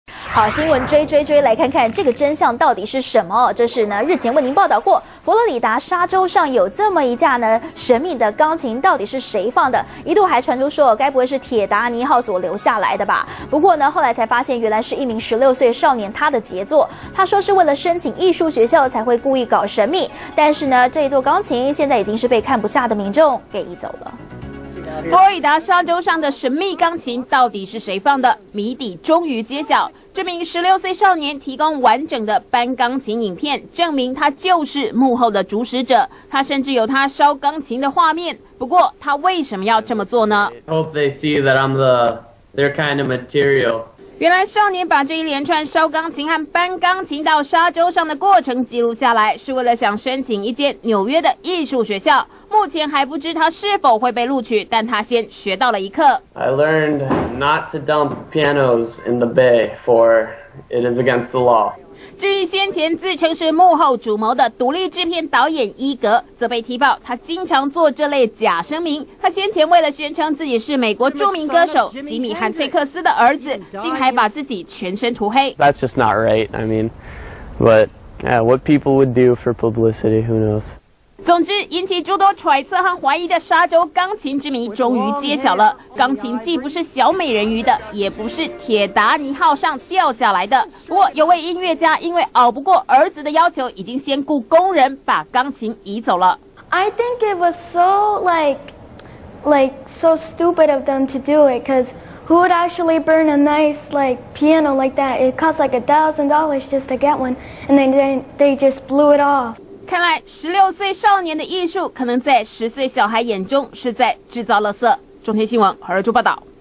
沙洲神秘鋼琴解謎 16歲少年藝術創作
中天新聞》美國邁阿密比斯肯灣一處小沙洲，出現一架小型神謎三角鋼琴引起世界關注，大家都想知道這架鋼琴是怎麼來的，原來是16歲的哈林頓和父親一起完成的惡作劇，目的是計畫拍攝成影片並拿來申請藝術學院。